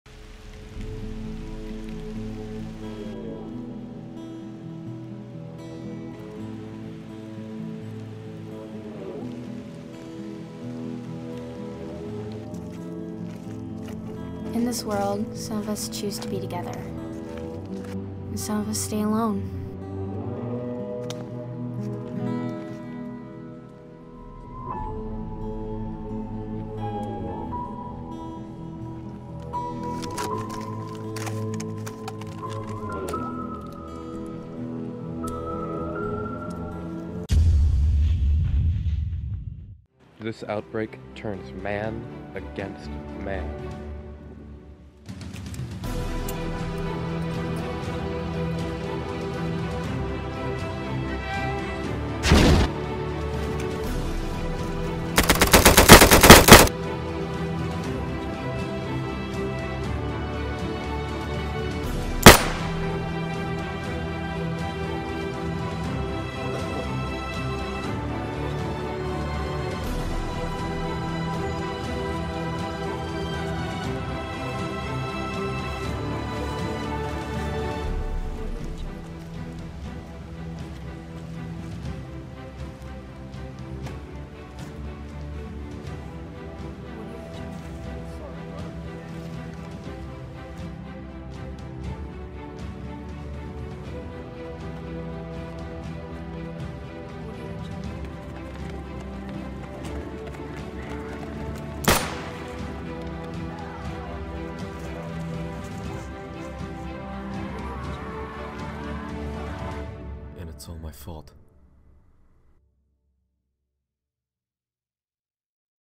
The OutBreak Trailer
This is the beginning of a new series we like to call The OutBreak. We will be releasing the first episode as soon as possible. We all have been very busy with school and our social lives also some of us have been sick. This video was entered in the Rob Dyke So you wanna be a youtuber contest and sadly did not win. Hopefully next year we will have a better chance.